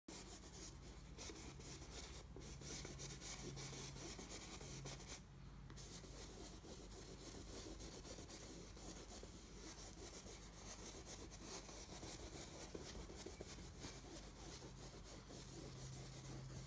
In The Artist Studio with Omar Obaid
Omar Obaid is one of Britain's fastest-growing artists. He creates original abstract paintings from his studio in London and also works on commissions and projects for private and commercial clients including interior designers, property developers and architects.

Here is a close-up shot of Omar painting an acrylic on canvas large artwork in his studio.